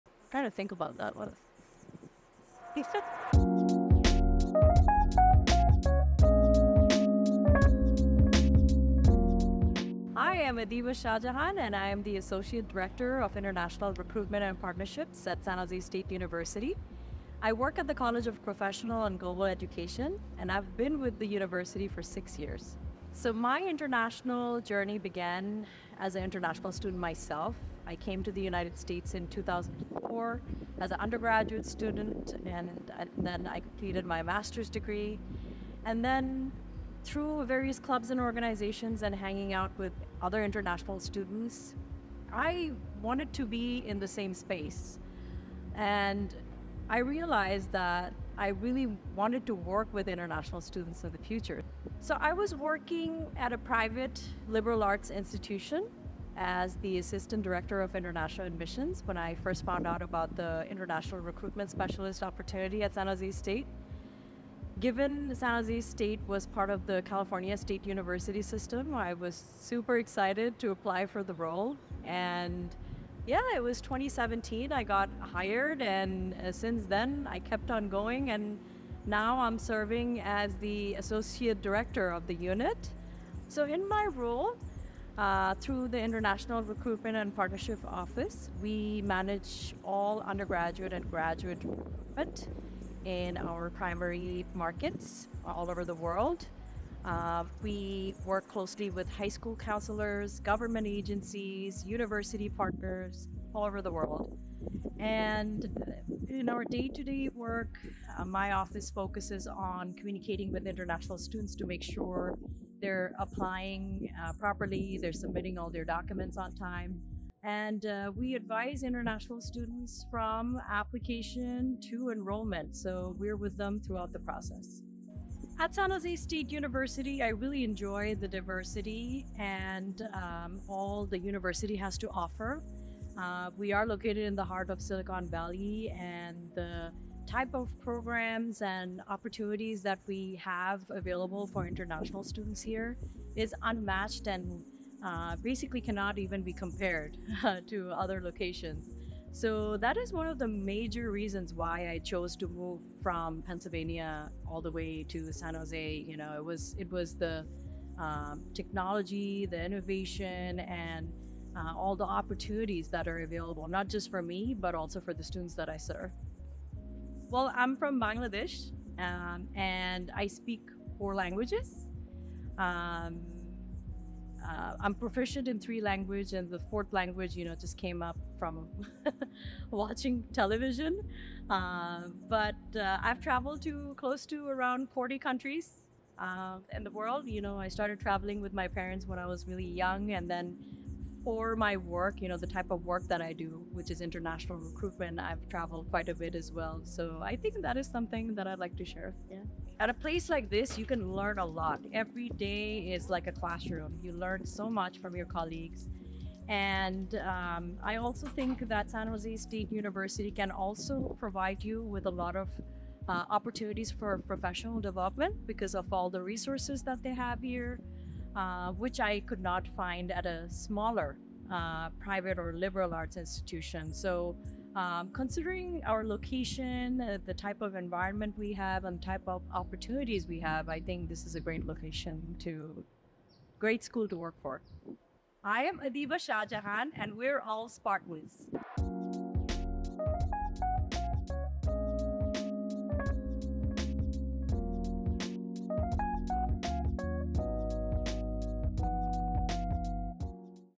We Are All SPARTANS! - Adiba Shahjahan
Adiba Shahjahan, Associate Director of International Recruitment and Partnerships in the College of Professional and Global Education, talks about their SJSU experience.

Video Production: Holly Quinones and Abeljohn Burke-Raymond